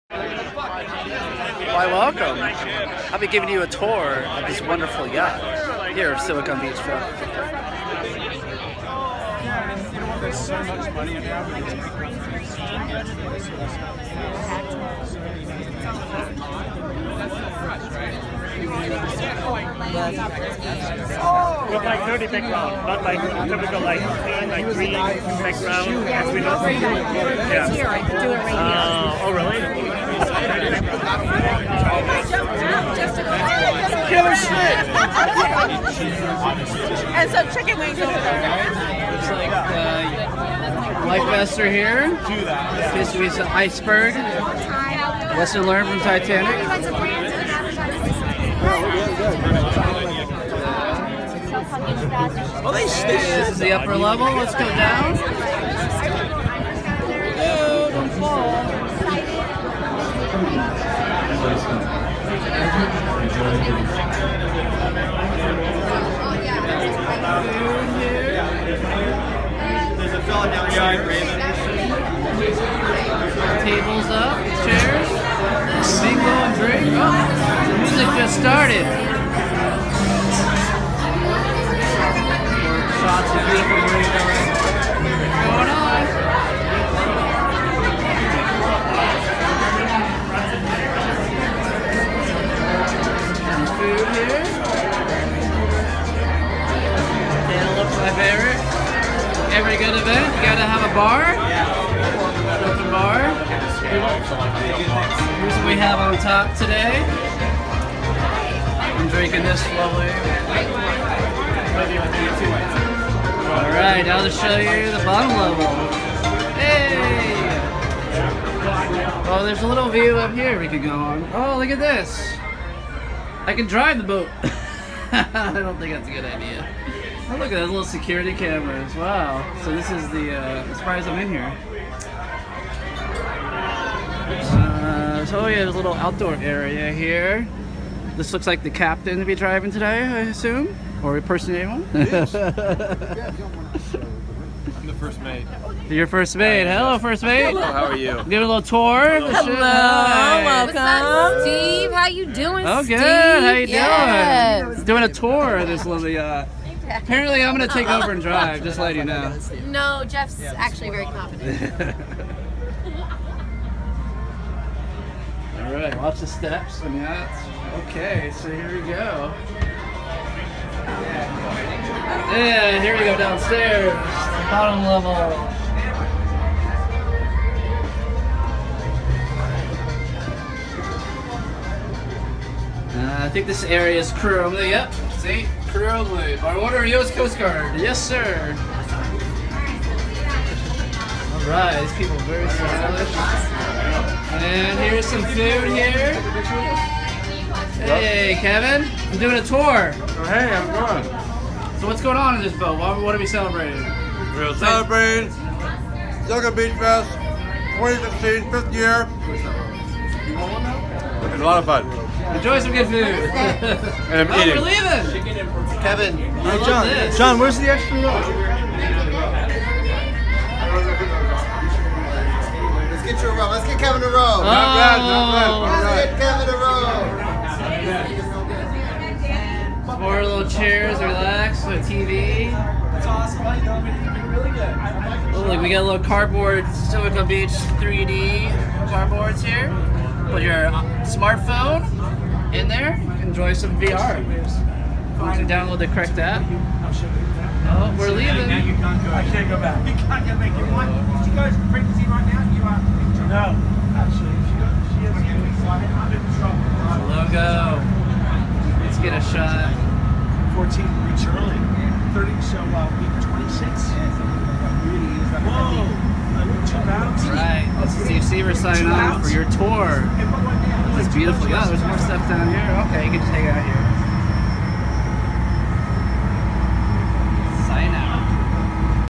Silicon Beach Fest VIP Yacht After Party
Silicon Beach Festival held a VIP after party for sponsors, speakers, & VIP guests. We got to board at the FantaSea Dock in Marina Del Ray with warm welcomes from the crew & captain of the yacht. We got to enjoy appetizers and beverages aboard the three-level FantaSea Yacht. The boat set sail for a cruise around the marina for spectacular views of the sunset & harbor.